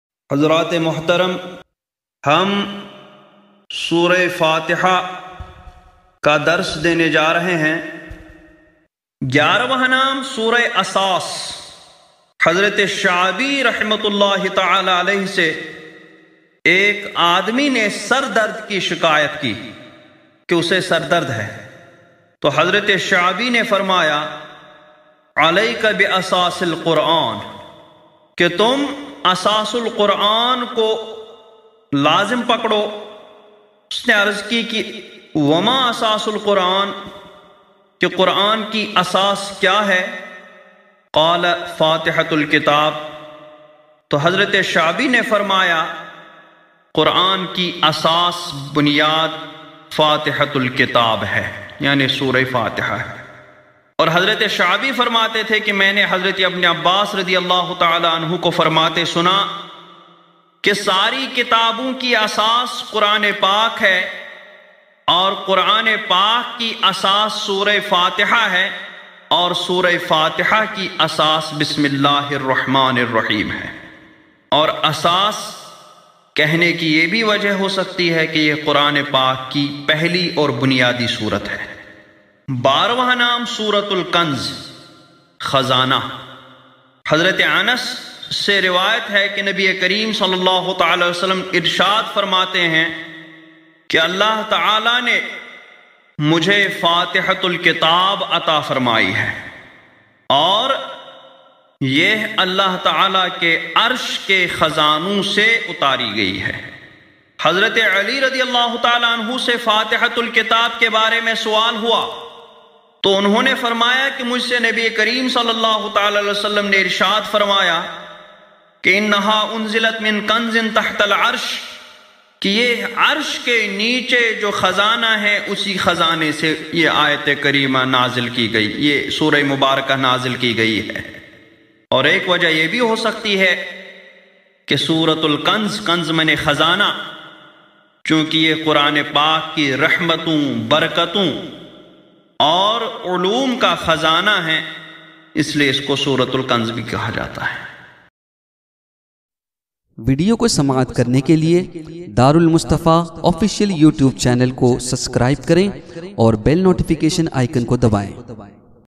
Suah Fatiha Ko Suratul Asaas Kyu Kehte Hain? Mufti Aashiq Husain
Tafseer e Quran :
By Khalifa e Huzur TajushShariah Daamad e Huzur Qaid e Millat Hazrat Allamah Mufti Ashiq Husain Kashmiri Sahib Qibla.

Bafaizan e Karam Sarkar TAJUSHSHARIAH علیہ الرحمہ
Ba Nazre Inayat Huzur QAAID E MILLAT مدظلہ النوراني